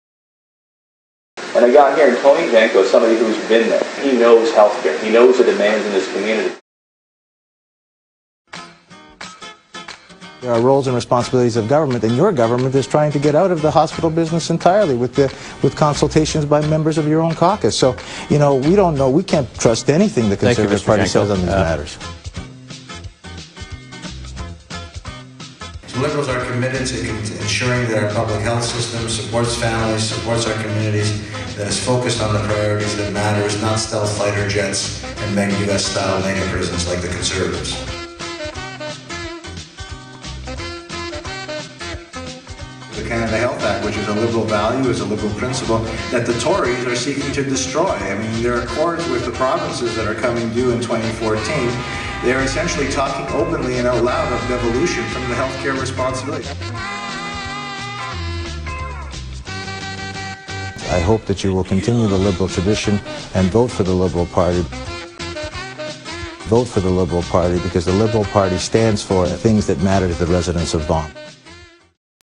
Tony Genco knows healthcare: Tim Hudak
Tim Hudak defers to Phony Tony on healthcare.

Listen to Tony, vote Liberal.